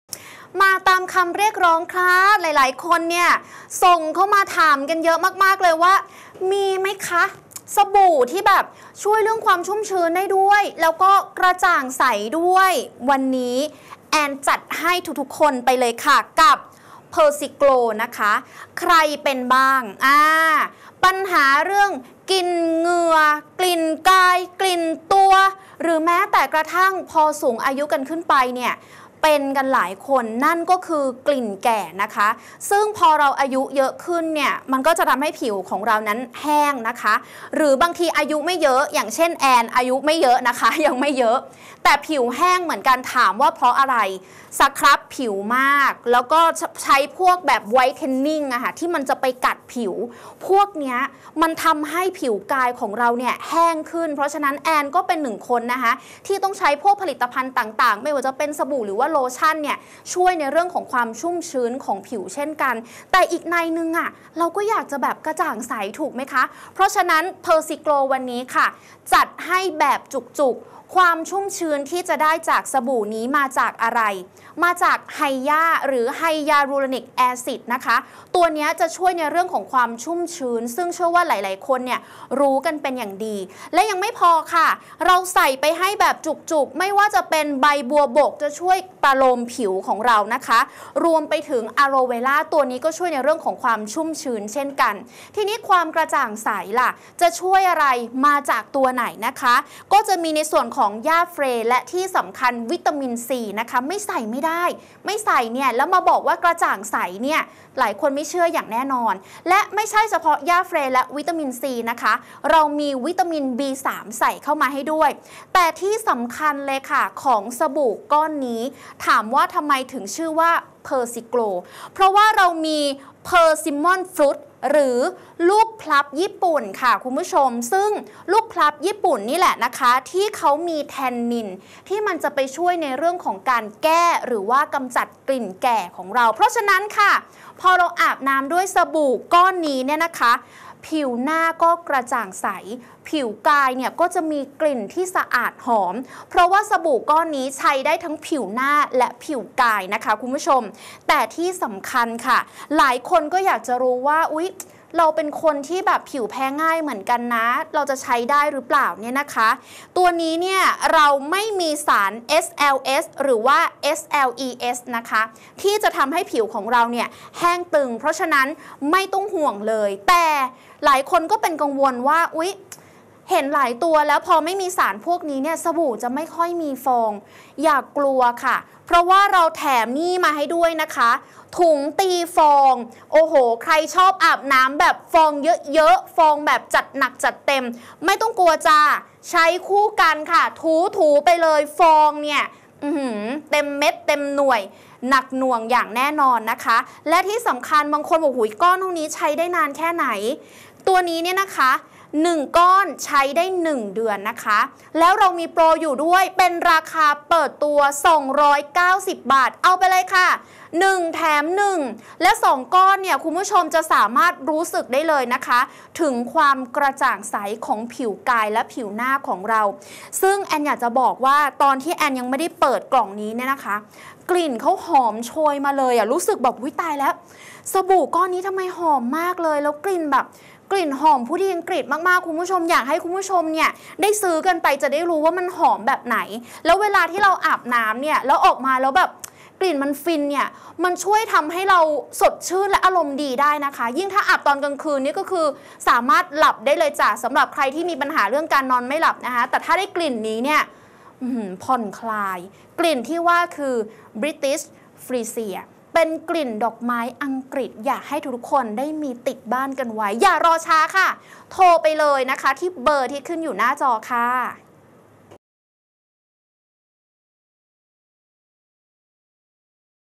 โปรโมชั่น สบู่ PersiGlow HYA SOAP ซื้อ1 แถม 1 ตั้งแต่วันนี้ถึงสิ้นเดือน!
สบู่ PersiGlow HYA SOAP ซื้อ1 แถม 1 ตั้งแต่วันนี้ถึงสิ้นเดือน!
1 ก้อนราคา 290บาท โปรโมชั่นวันนี้ถึงสิ้นเดือน หรือจนกว่าของจะหมด